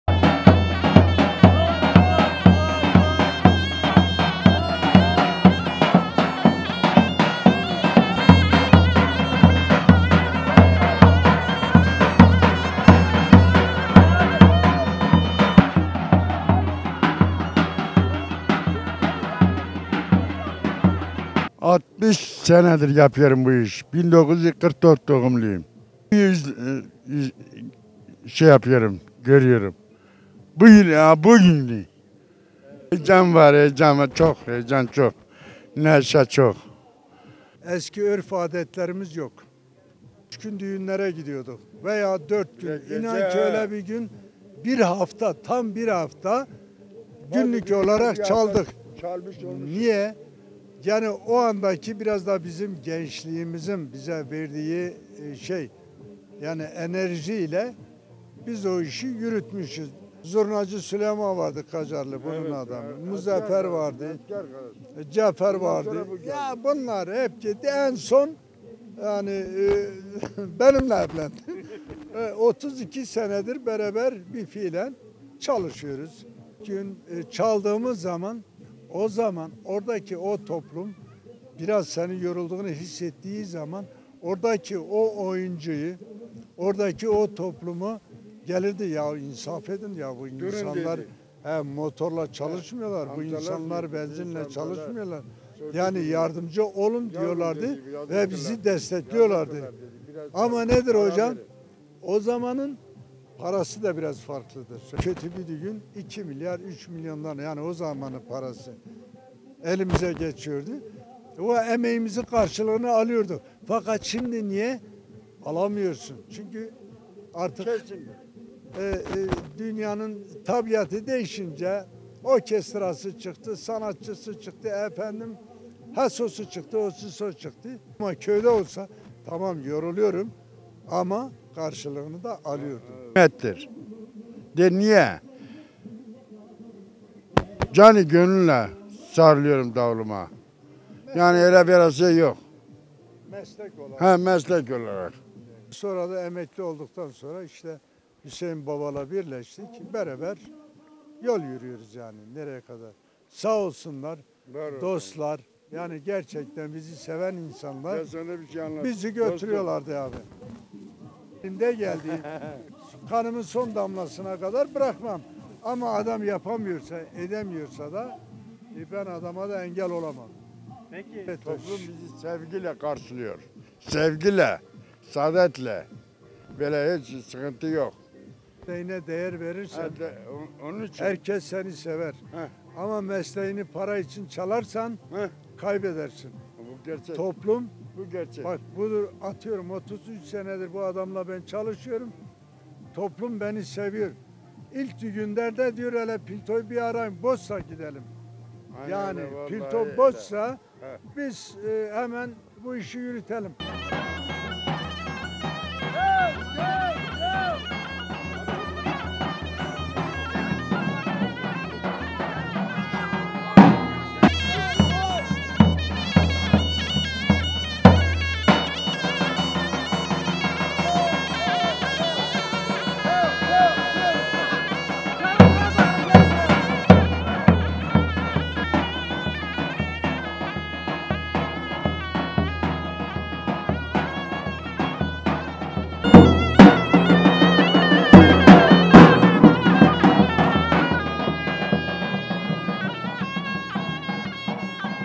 Davulcu Pilto, yıllara meydan okuyor
PİRHA- Dersim'de yıllara meydan okuyan Davulcu Pilto ve Zurnacı Fırat, ilk günün heyecanıyla düğünlere gittiklerini belirterek, toplumun da kendilerine değer vermesinin onur verici olduğunu söylüyor.